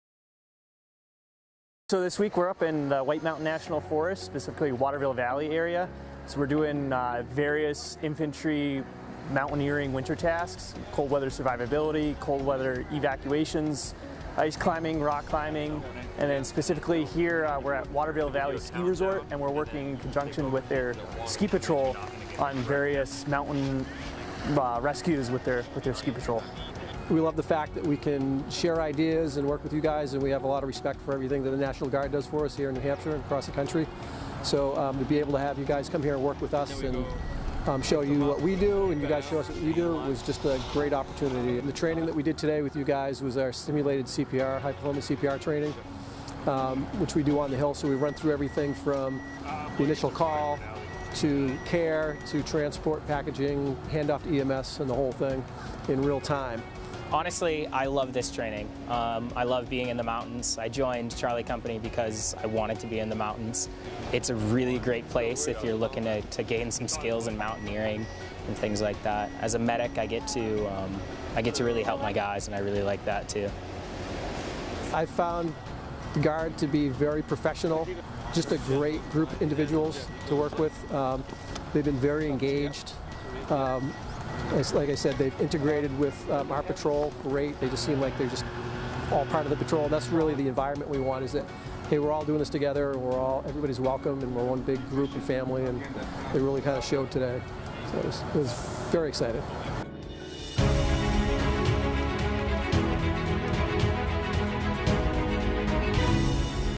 New Hampshire National Guardsmen conduct cold-weather training
New Hampshire Army National Guard Soldiers from Charlie Company, 3rd Battalion, 172nd Infantry Regiment (Mountain) conduct winter medical and mountaineering training from Jan. 5 through 8, in Waterville Valley, NH. (Video by Sgt. Bei Simmons)